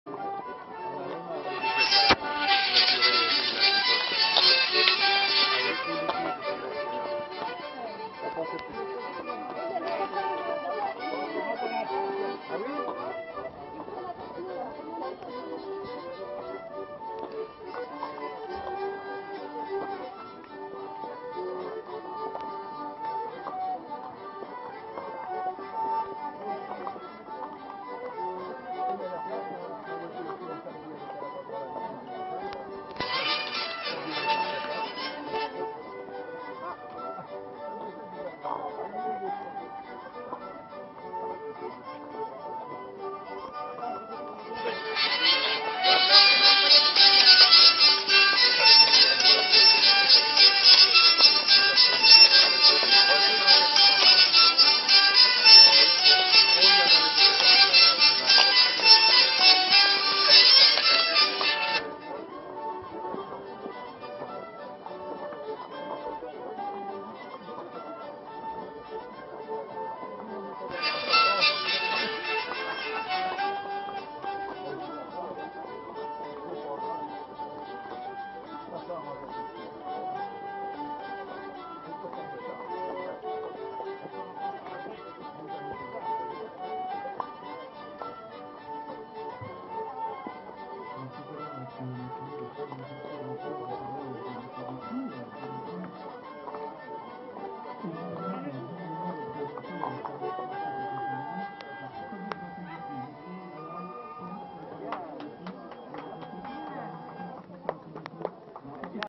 Living In France: Traditional French Dance
This is a typical French Market in the Berry region of France.  Notice the yellow sign "escargots a emporter" meaning snails for take away.